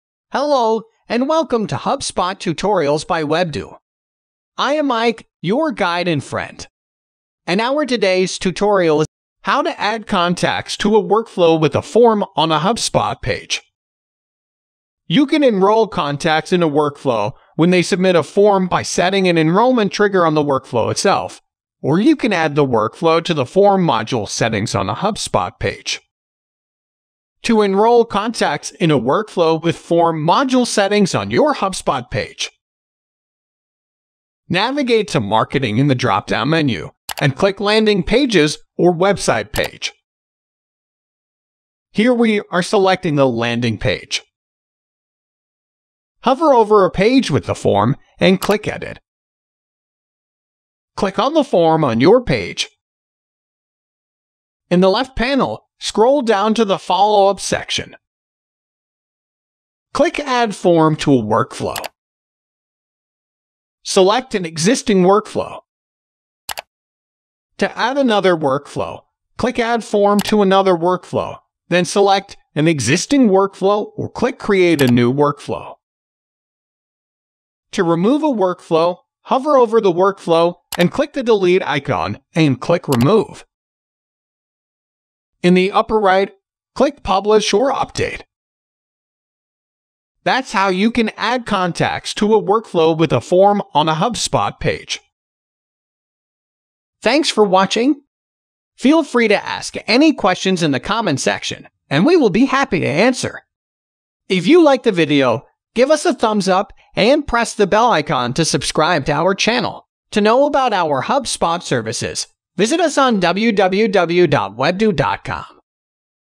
How to add contacts to a workflow with a form on a HubSpot page
In this HubSpot tutorial, we will walk you through how to add contacts to a workflow with a form on a HubSpot page.

You can enroll contacts in a workflow when they submit a form by setting an enrollment trigger on the workflow itself, or you can add the workflow to the form module settings on a HubSpot page.

Steps to follow:

Step 1. To enroll contacts in a workflow with form module settings on your hubspot page.

Step 2. Navigate to marketing in the dropdown menu, and click Landing pages or Website page.

Step 3. Here we are selecting the landing page.

Step 4. Hover over a page with a form and click Edit.

Step 5. Click on the form on your page.

Step 6. In the left panel, scroll down to the Follow-up section.

Step 7. Click Add form to a workflow.

Step 8. Select an existing workflow.

Step 9. To add another workflow, click Add form to another workflow, then select an existing workflow or click Create a new workflow.

Step 10. To remove a workflow, hover over the workflow and click the delete icon and click remove.

Step 11. In the upper right, click Publish or Update.

That’s how you can add contacts to a workflow with a form on a HubSpot page.